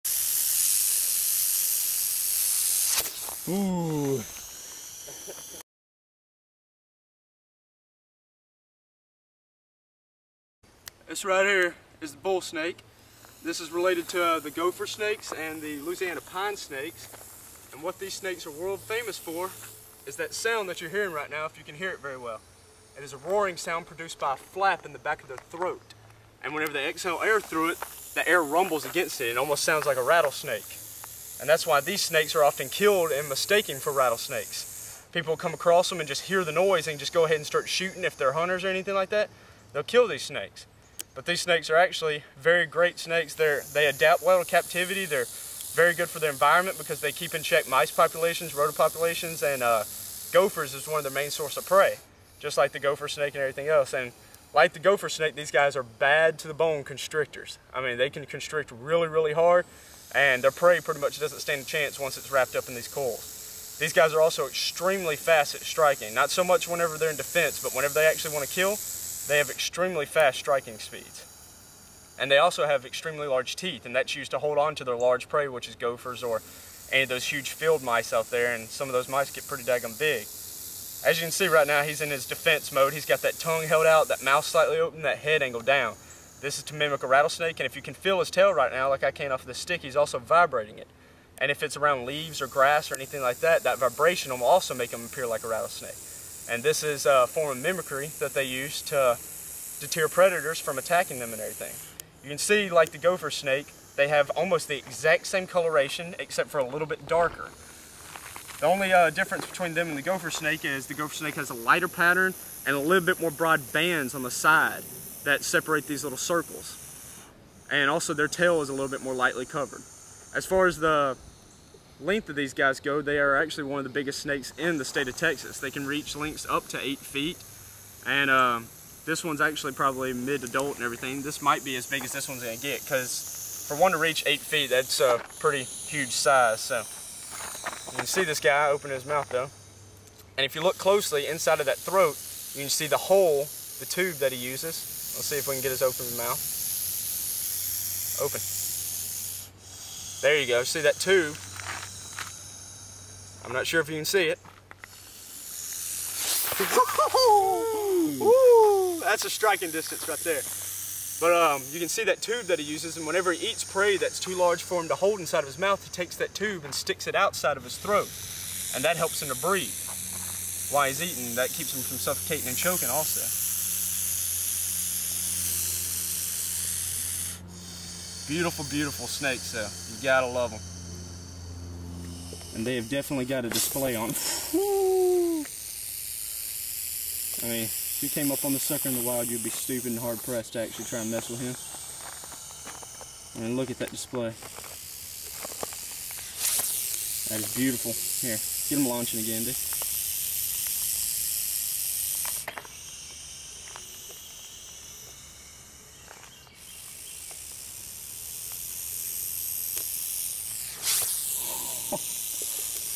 Bullsnake
My name is Orry Martin: The Texas Snake Hunter.  This is the Bullsnake which is a close relative to the Sonoran Gopher snake of Texas.  These snakes are famous for their ability to exhale air past a flap in their throats to produce a loud hissing/rattle sound.  All pictures were taken by me on my Nikon D200.